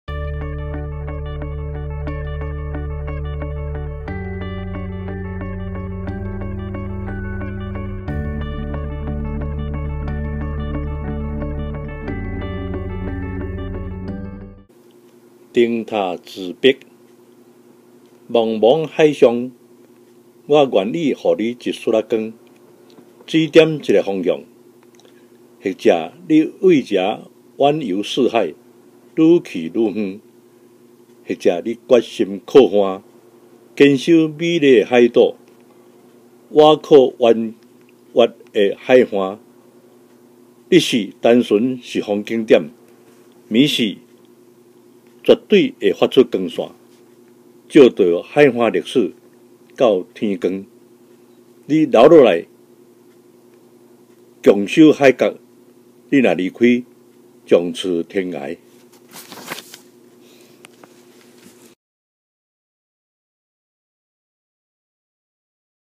الشاعر التايواني ليكوين-شاين Balcony Poetry Festival 2020 - Lee Kuei-shien ( Taiwan
Balcony Poetry Festival 2020
House of poetry

ليكوين-شاين (شــاعرٌ من تايوان)

ولد لي كوي- شاين عام 1937. تقاعد من رئاسة مؤسسة الثقافة والفنون في تايوان ، ويشغل في الوقت الحالي منصب نائب رئيس الحركة الشعرية قي "موندو" . نشر سبعة وعشرين مجموعة شعرية ، تُرجم منها كتاب "ساعة الغسق" للإنجليزية والمنغولية والرومانية والروسية والإسبانية والفرنسية والكورية والبنجالية والألبانية والتركية والمقدونية والألمانية والصربية والعربية .

مونولوج على مقربةٍ من الفنار

ليكوين-شاين
 (شــاعرٌ من تايوان)

في حضرة البحرالشاسع
أريد أن أمنح كبقعةً من الضوء
تقود إلى وجهةٍ بعينها.
فربما أردتَ الرحيل إلى أماكن أخرى
هناك إلى البعيد ، البعيد
وربما آثرتَ البقاء هنا
على شاطئ هذه الجزيرة المدهشة
مؤاخياً هذا الساحل العاصف.
على ضوء النهار،
ربما بدت مجرد مشهدٍ بسيط
أما تحت جنح الليل ، فإنها عادةً ما تبث شعاعاً من النور
يضيء تاريخ الساحل النهريّ
حتى بزوغ الفجر
إنبقيتَ ،
سنكون رفيقين على متن هذه الجزيرة،
أما إن قررتَ الرحيل،
فليس أمامنا سوى أن نفترق
إلى الأبد.

ـــــــــــــــــــــــــــــــــــــ
 (* ترجمة : حمدي الجابري

Lee Kuei-shien
 (Taiwan)

Lee Kuei-shien（b. 1937）has retired from the chairman of National Culture and Arts Foundationof Taiwan, presently the Vice President of MovimientoPoetasdelMundo. He published27 books of poetry, in which “The Hour of Twilight” has been translated into English, Mongol, Romanian, Russian, Spanish, French, Korean, Bengali, Albanian, Turkish, Macedonian, German, Serbian and Arabic languages.

“Monologue by the Lighthouse”

On the vast sea
I wish to give you a spot of light
indicating a certain direction.
Perhaps you may depart for everywhere
farther and farther away
or you may decide to moor on the shore
staying together with this beautiful island
along the winding coast.
In the daytime, may be just a simple scenery
at night, it definitely emits a brilliant ray
illuminating the history of seacoast
until dawn.
If you stay, we accompany on island.
If you leave, we separate forever.